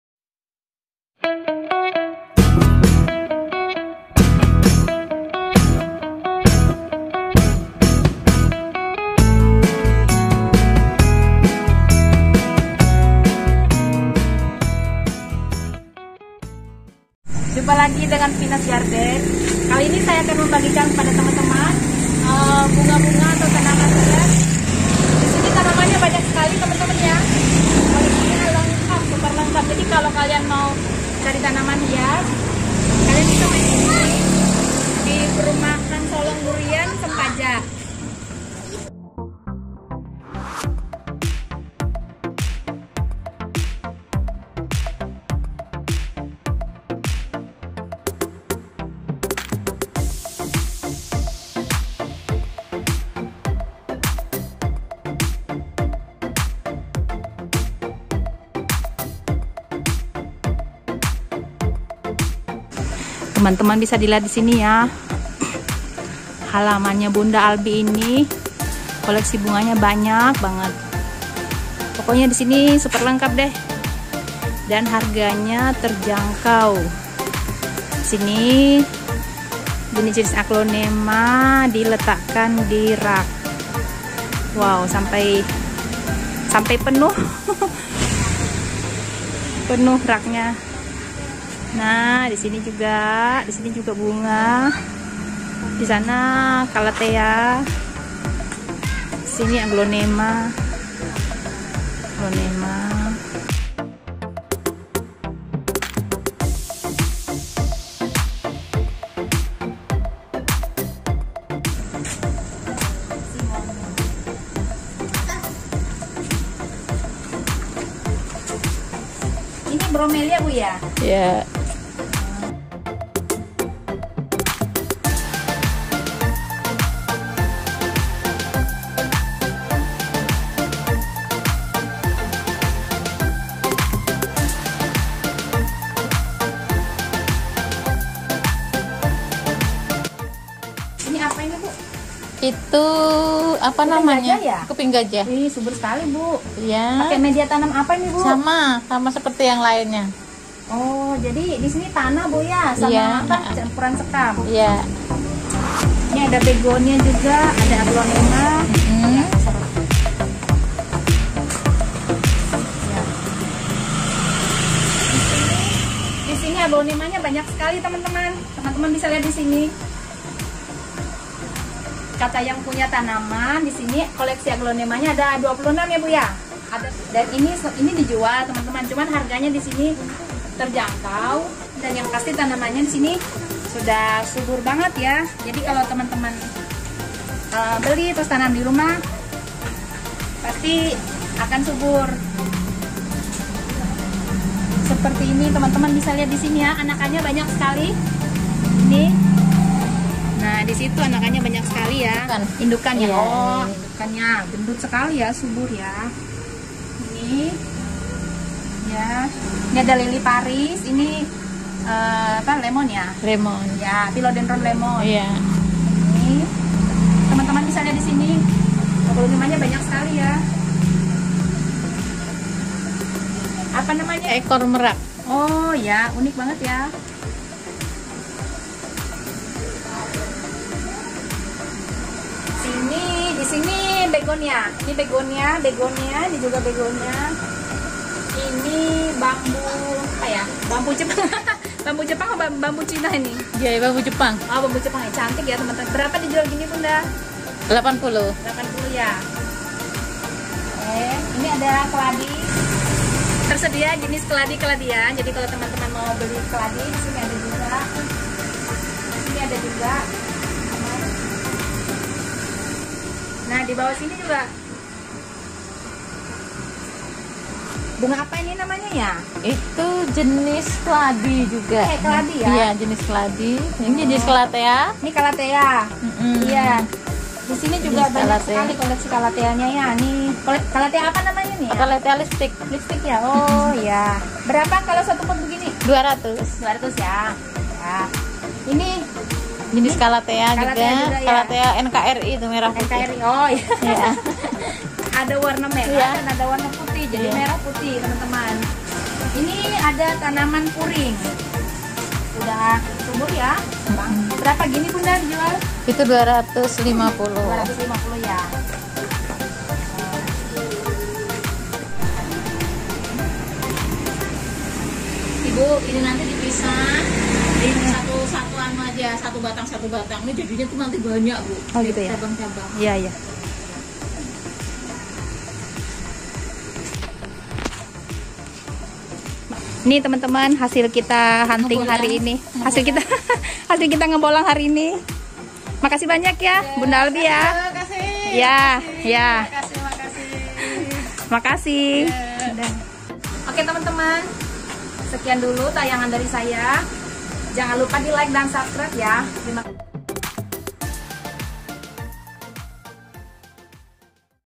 Hunting tanaman hias di kota SAMARINDA
kegiatan saya sore ini adalah hunting tanaman hias keliling kota Samarinda dan saya menemukan salah satu tempat yang menjual beraneka ragam tanaman hias. harga yg ditawarkan bervariasi mulai dari harga 100 ribu kebawah hingga 400 ribuan. tanaman yg saya temukan di sini sangat subur karena perawatannya cukup intensif.

salam
Vina's Garden.